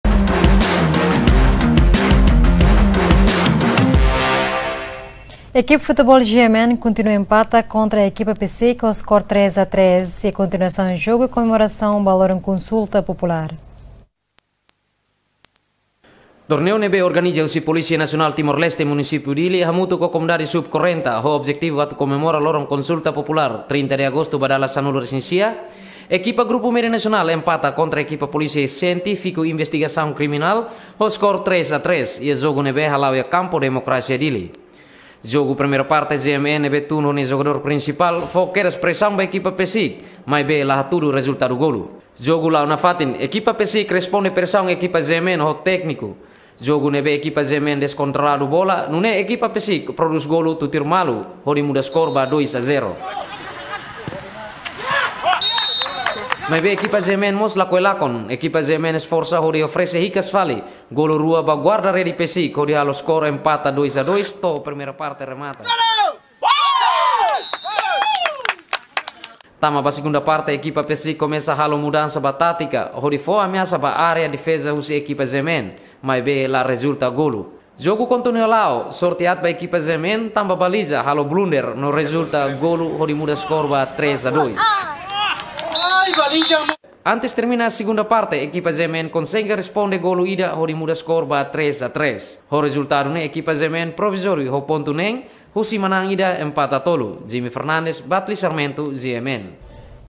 Ekipa PCIC Empata ho Ekipa GMN 3 - 3
Ekipa PCIC Empata ho Ekipa GMN 3 - 3 iha kontinuasaun jogu komemorasaun konsulta populár 2018